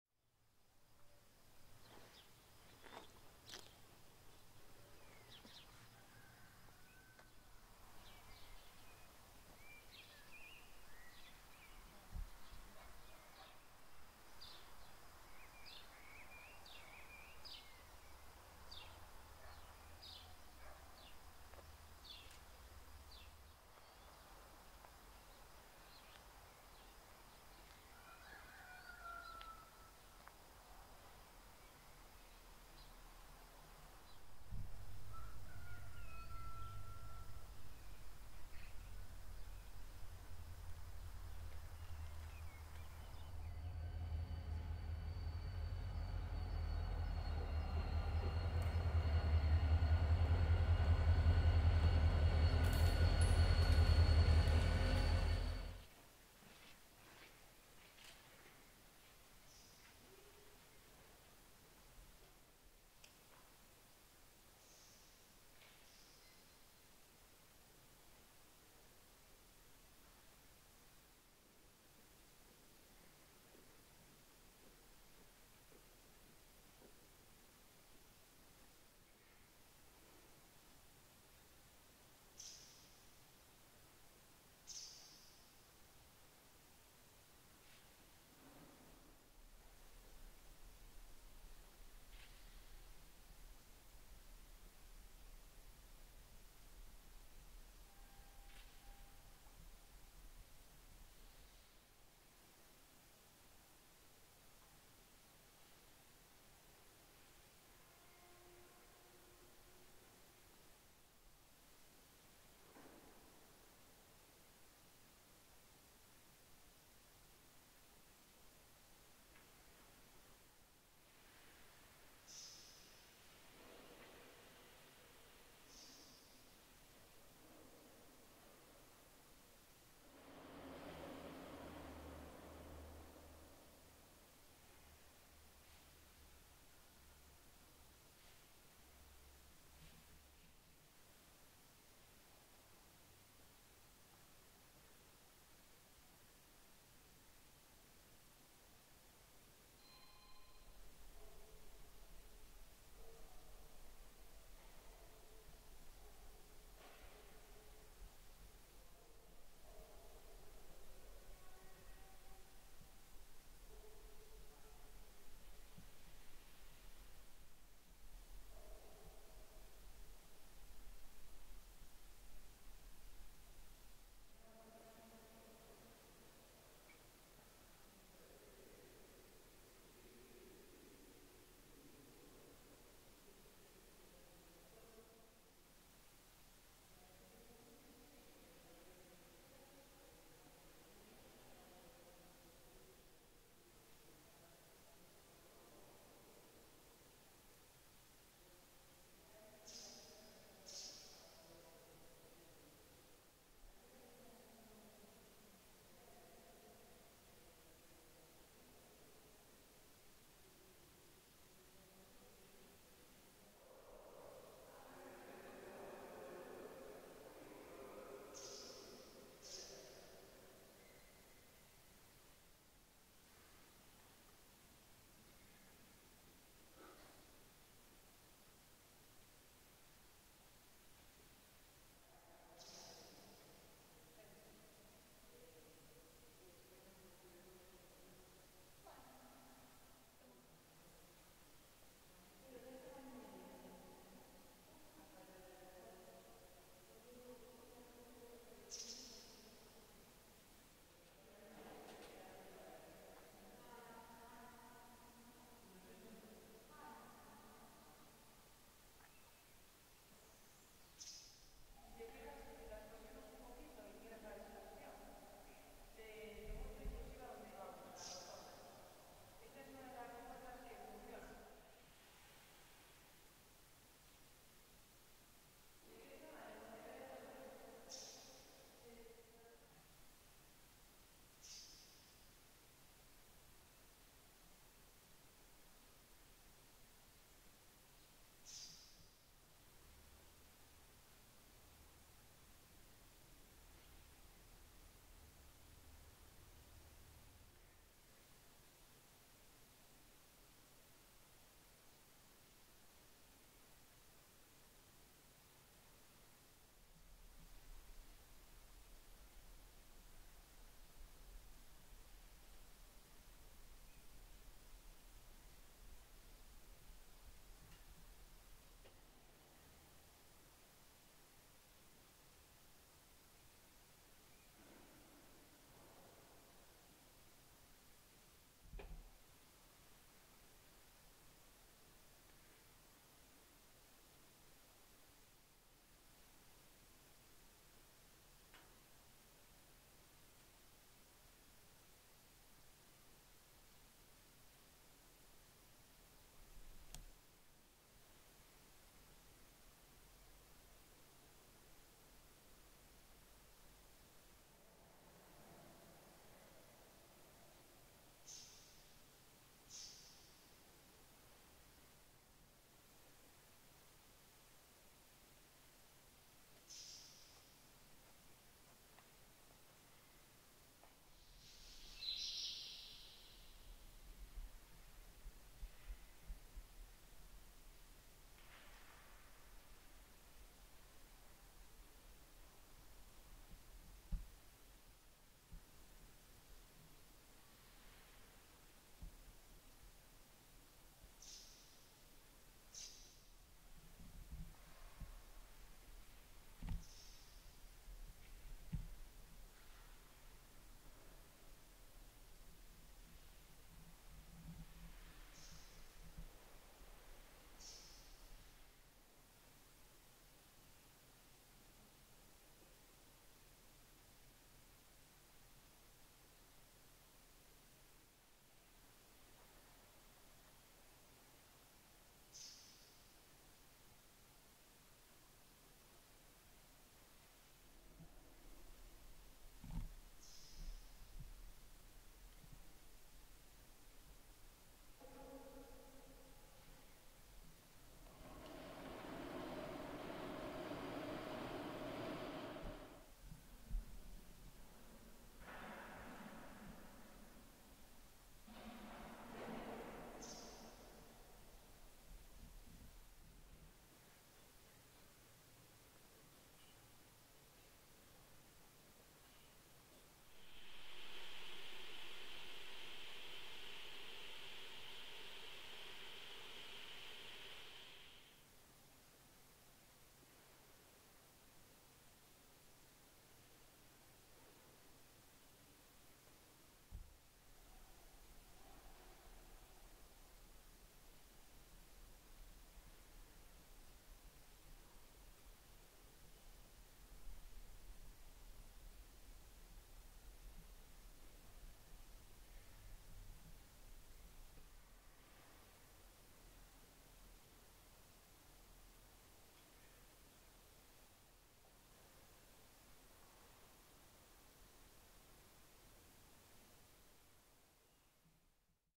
MUFERGA, Monforte de Lemos. 22/05/2019
Impresionante museo del ferrocarril, con piezas históricas perfectamente conservadas, e innumerables y carismáticos artefactos tecnológicos del pasado.